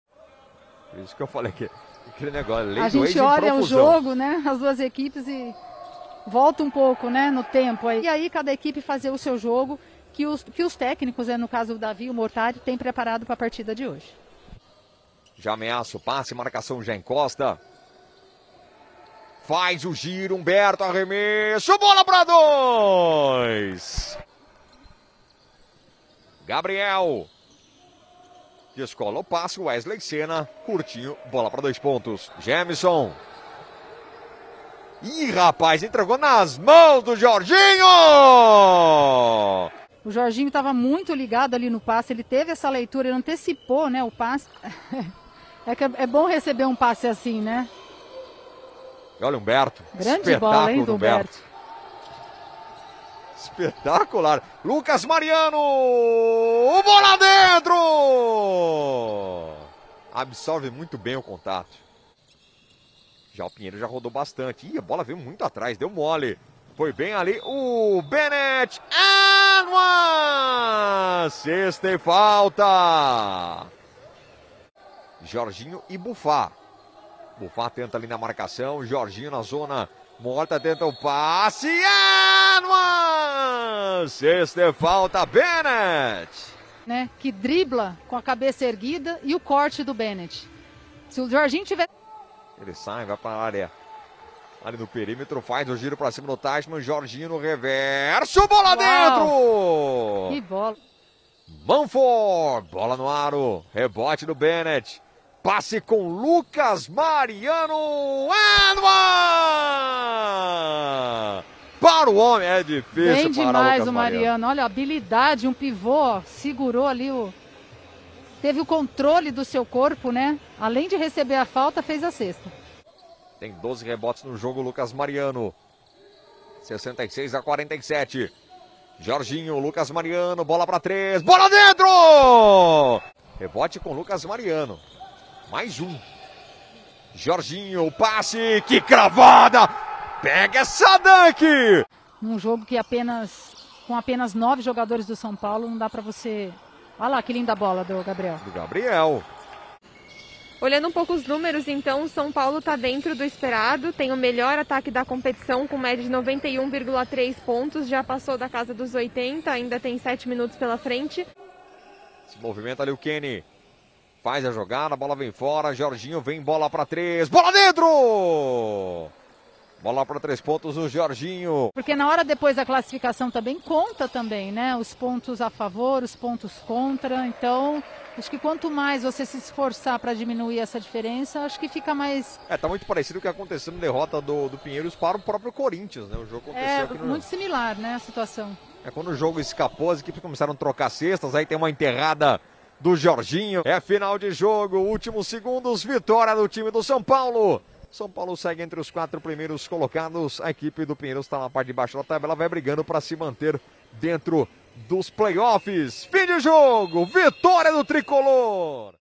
Melhores Momentos | São Paulo x Pinheiros | 10.02.2021
Confira os melhores momentos da partida entre São Paulo e Pinheiros